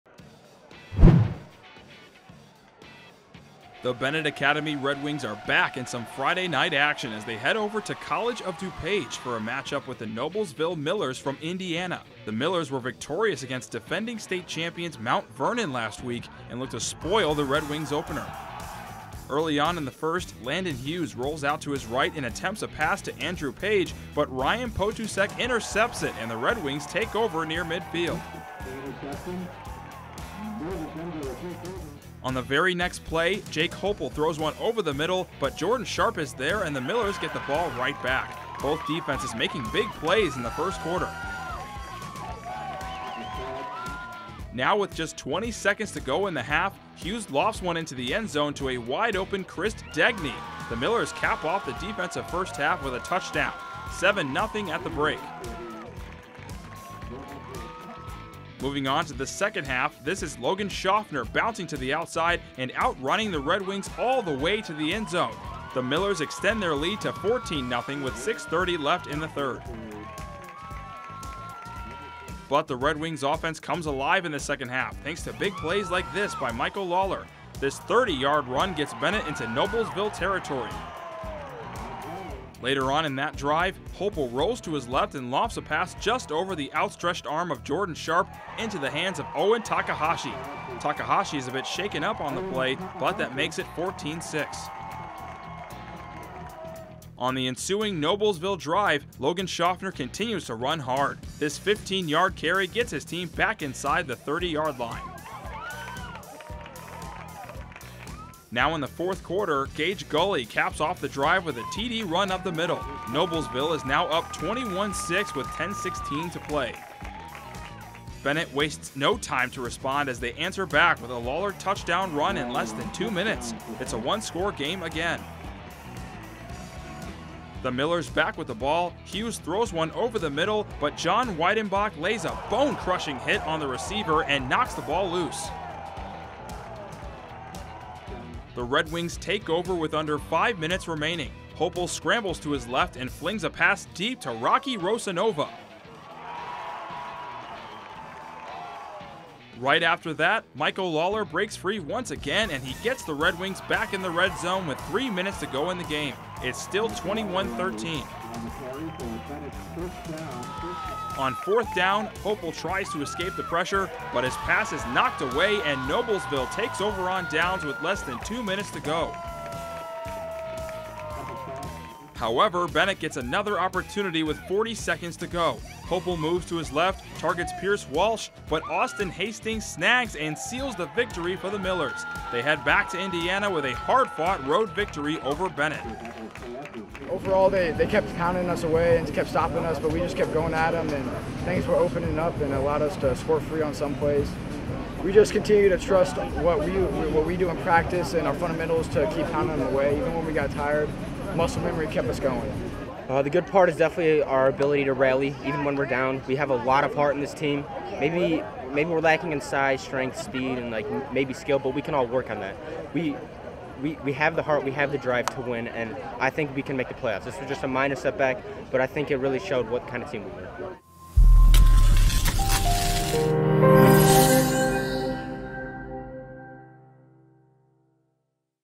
Football Noblesville vs. Benet Academy 08.26.22
Benet Academy football matches up with Noblesville in some Friday night action where the Millers get the victory over the Redwings.

The Benet Academy Redwings are back in some Friday night action as they head over to College of DuPage for a matchup with the Noblesville Millers from Indiana. The Millers were victorious against defending state champions Mt. Vernon last week, and look to spoil the Redwings’ opener.

First Quarter

Early on in the 1st, Landon Hughes rolls out to his right and attempts a pass to Andrew Page, but Ryan Potucek intercepts it and the Redwings take over near midfield.

On the very next play, Jake Hoeppel throws one over the middle, but Jordan Sharp is there and the Millers get the ball right back. Both defenses making big plays in the 1st quarter.

Second Quarter

Now with just 20 seconds to go in the half, Hughes lofts one into the end zone to a wide-open Christ Degni. The Millers cap off the defensive first half with a touchdown. 7-0 at the break.

Third Quarter

Moving on to the second half, this is Logan Shoffner bouncing to the outside and outrunning the Redwings all the way to the end zone. The Millers extend their lead to 14-0 with 6:30 left in the 3rd.

But the Redwings offense comes alive in the second half, thanks to big plays like this by Michael Lawler. This 30-yard run gets Benet Academy football into Noblesville territory.

Later on in that drive, Hoeppel rolls to his left and lofts a pass just over the outstretched arm of Jordan Sharp into the hands of Owen Takahashi. Takahashi is a bit shaken up on the play, but that makes it 14-6.

On the ensuing Noblesville drive, Logan Shoffner continues to run hard. This 15-yard carry gets his team back inside the 30-yard line.

Fourth Quarter

Now in the 4th quarter, Gage Gulley caps off the drive with a TD run up the middle. Noblesville is now up 21-6 with 10:16 to play.

Benet wastes no time to respond as they answer back with a Lawler touchdown run in less than 2 minutes. It’s a one-score game again.

The Millers back with the ball, Hughes throws one over the middle, but John Weidenbach lays a bone-crushing hit on the receiver and knocks the ball loose.

The Redwings take over with under 5 minutes remaining. Hoeppel scrambles to his left and flings a pass deep to Rocky Rosanova.

Right after that, Michael Lawler breaks free once again and he gets the Redwings back in the red zone with 3 minutes to go in the game. It’s still 21-13.

On fourth down, Hoeppel tries to escape the pressure, but his pass is knocked away and Noblesville takes over on downs with less than 2 minutes to go.

However, Benet gets another opportunity with 40 seconds to go. Hoeppel moves to his left, targets Pierce Walsh, but Austin Hastings snags and seals the victory for the Millers. They head back to Indiana with a hard-fought road victory over Benet.

ABOUT US
Naperville Community Television (NCTV17) broadcasts on Channel 17 in Naperville, IL (Comcast & WOW!) Programming is also available via simulcast and video on-demand